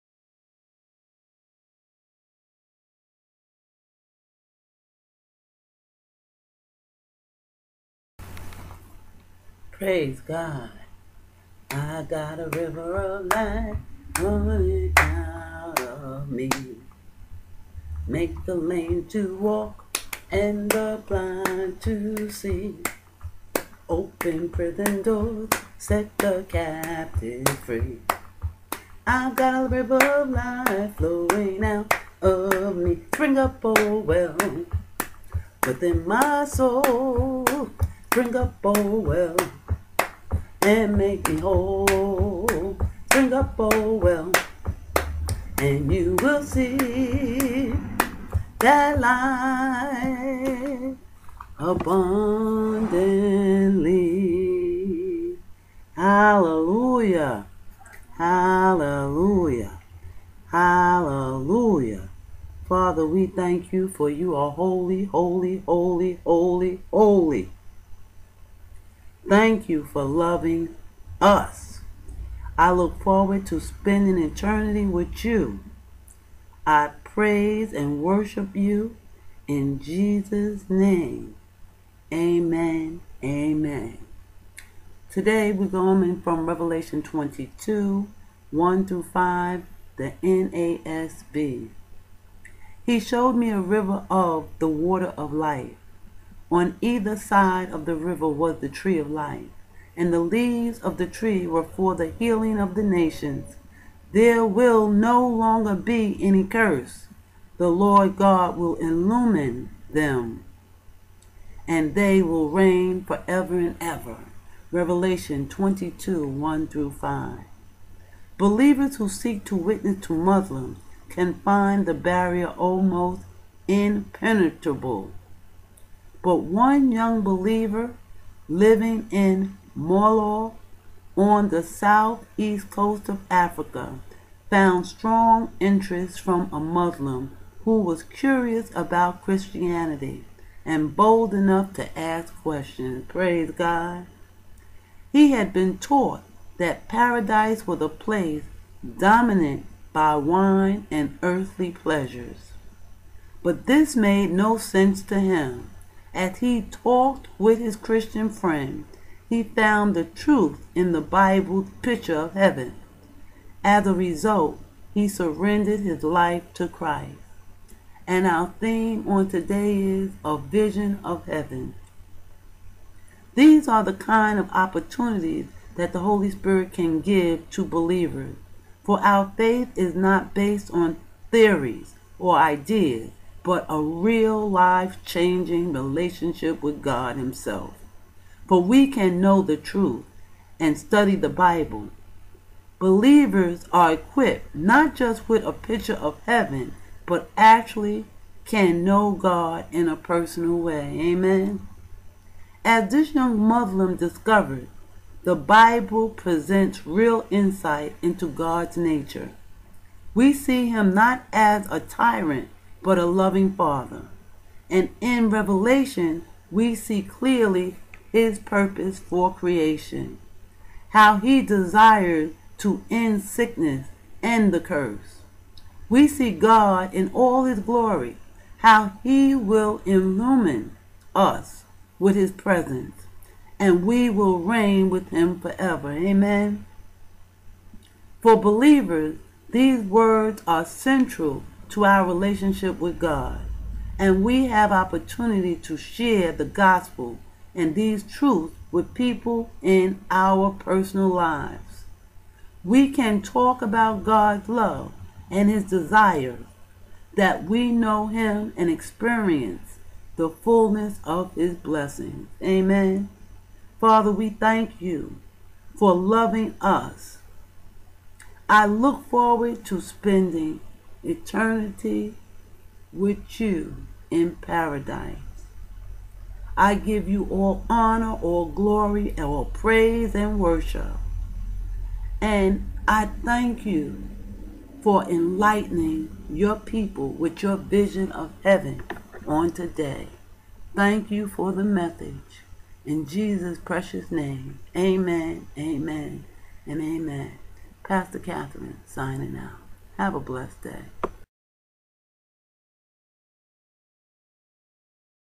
482. Bible Study: A Vision of Heaven
Happy Valentine Day 2/14/15 to all. We all need a vision of heaven and how much Yah loves us! I want to say Happy Valentine to Yah, Yeshua & The Holy Spirit.